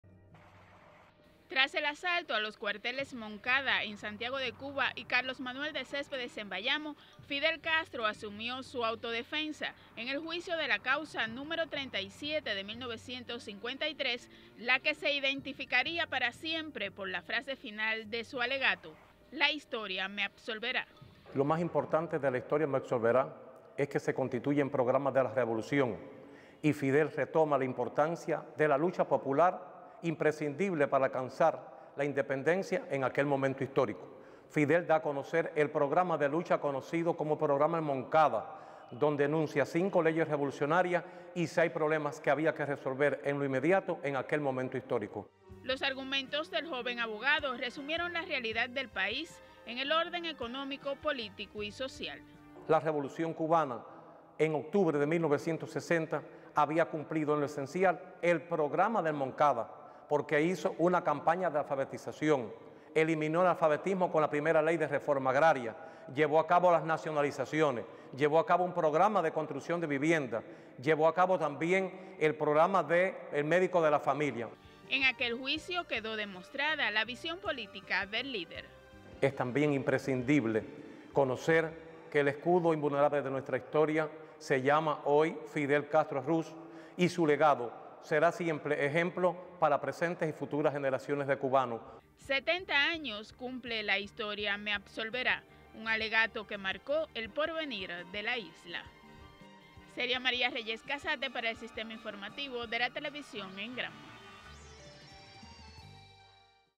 “La Historia me Absolverá”
En 1953 el joven abogado Fidel Castro protagonizó su autodefensa en el juicio por el asalto a los cuarteles Moncada y Carlos Manuel de Céspedes, conocida como “La Historia me Absolverá”.  Cuba conmemora este 16 de octubre  el  aniversario 70 del auto_alegato del líder histórico de la Revolución cubana por los hechos el 26 de julio de ese propio año.